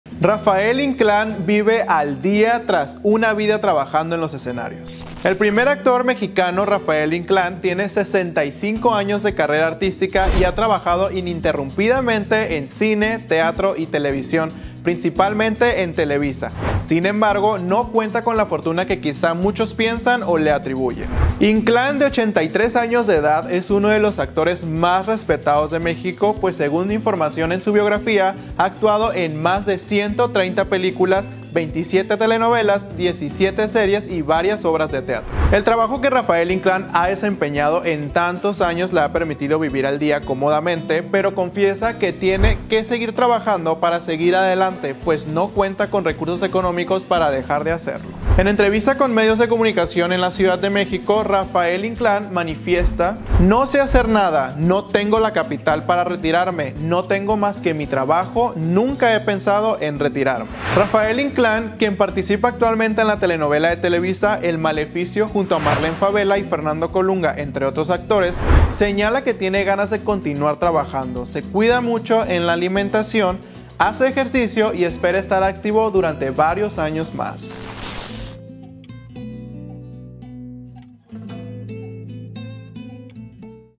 Rafael Inclán revela por qué vive AL DÍA y NO tiene DINERO
Rafael Inclán, una vida en los escenarios trabajando, revela por qué vive AL DÍA y no tiene dinero
El primer actor mexicano Rafael Inclán tiene 65 años de carrera artística y ha trabajado ininterrumpidamente en cinte, teatro y televisión, principalmente en Televisa, sin embargo, no cuenta con la fortuna que quizá muchos piensan o le atribuyen.
Rafael Inclán, de 83 años de edad, es uno de los actores más respetados de México, pues según información en su biografía, ha actuado en más de 130 películas, 27 telenovelas, 17 series y varias obras de teatro.
El trabajo que Rafael Inclán ha desempeñado en tantos años le ha permitido vivir al día, cómodamente, pero confiesa que tiene que seguir trabajando para salir adelante, pues no cuenta con recursos económicos para dejar de hacerlo.
En entrevista con medios de comunicación en Ciudad de México, Rafael Inclán manifiesta: “No sé hacer nada, no tengo capital para retirarme. No tengo más que mi trabajo. Nunca he pensado en retirarme” Rafael Inclán, quien participa actualmente en la telenovela de Televisa 'El Maleficio', junto a Marlene Favela y Fernando Colunga, entre otros actores, señala que tiene ganas de continuar trabajando, se cuida mucho en la alimentación, hace ejercicio y espera estar activo durante varios años más.